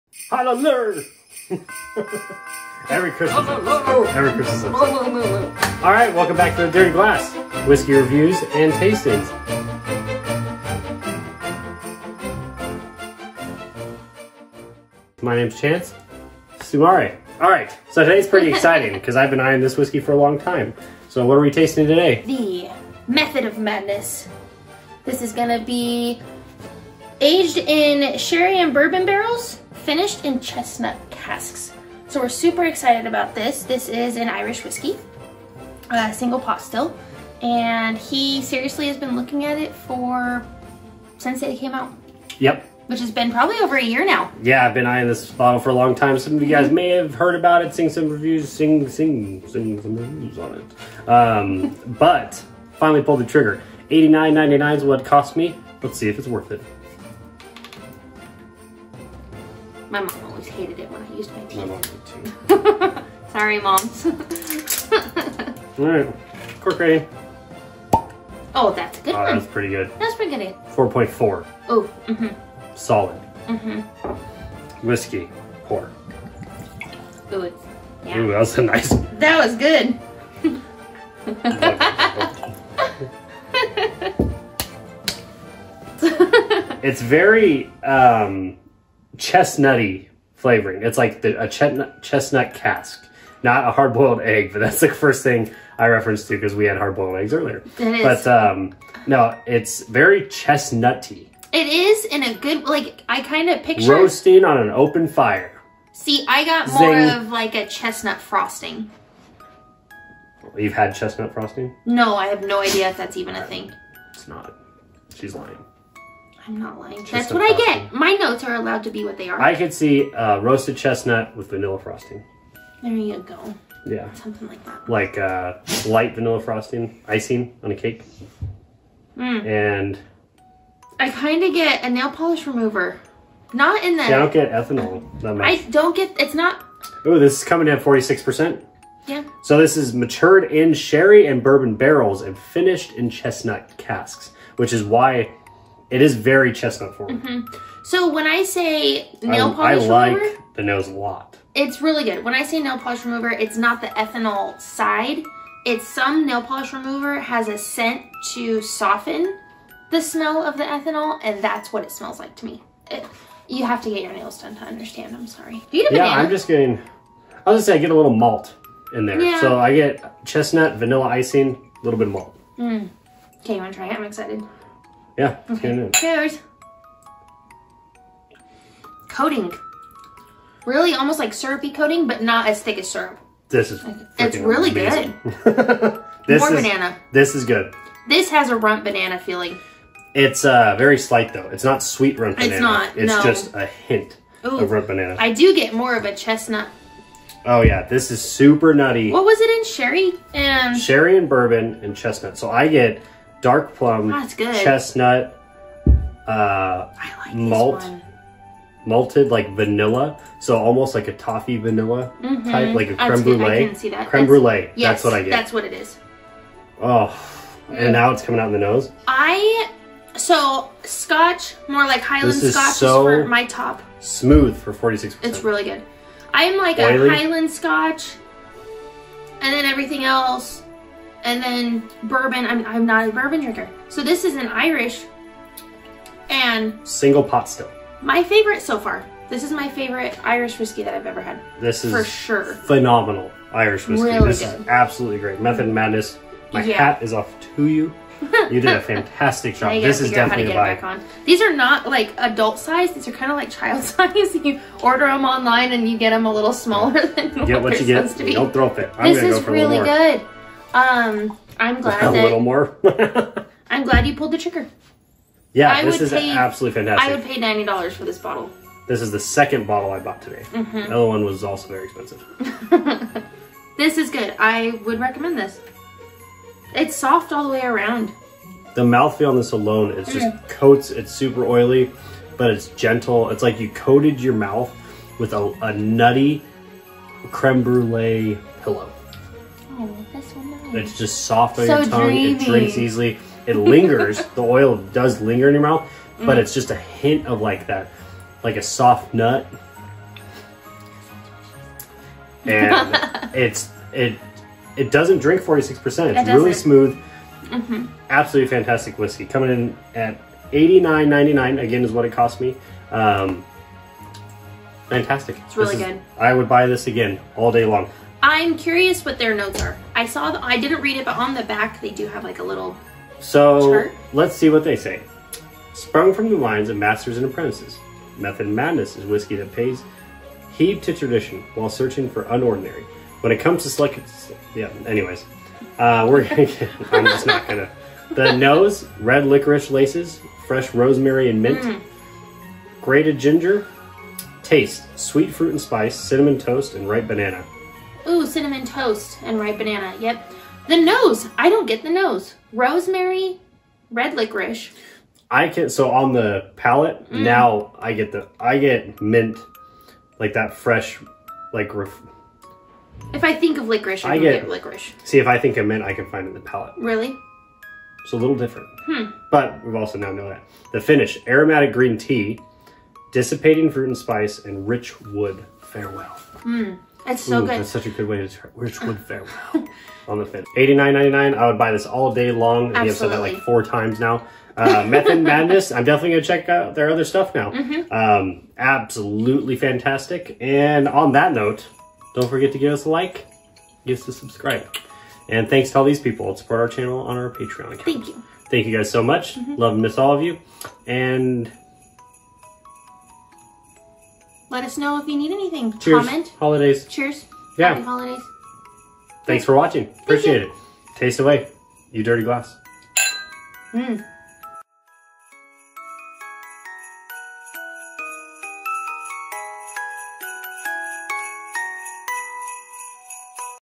Whiskey review #42. Method and Madness Single Pot Still Irish Whisky
We Review  Method and Madness Single Pot Still Irish Whisky!